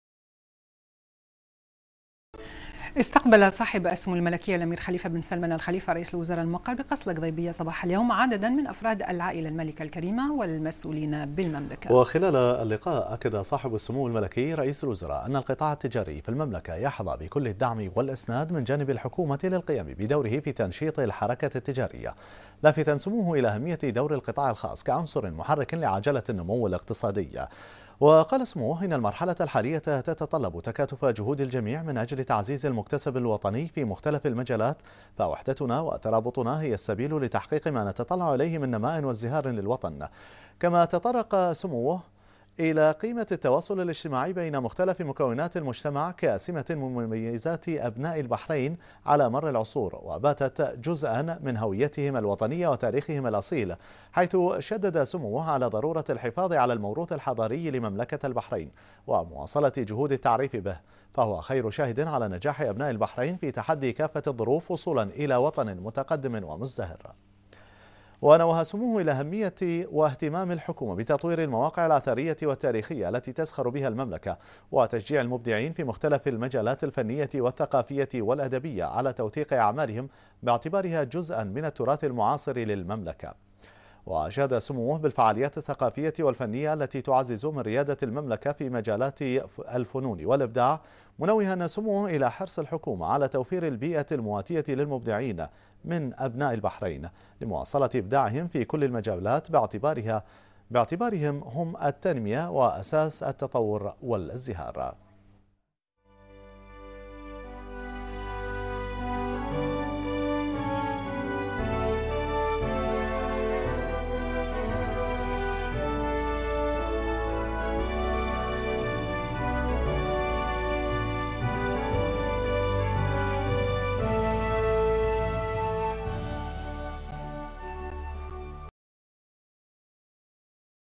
البحرين: سمو رئيس الوزراء يستقبل عدد من أفراد العائلة المالكة
المنامة في 24 يناير/بنا/ استقبل صاحب السمو الملكي الأمير خليفة بن سلمان آل خليفة رئيس الوزراء الموقر بقصر القضيبية صباح اليوم عددا من أفراد العائلة المالكة الكريمة والمسؤولين بالمملكة.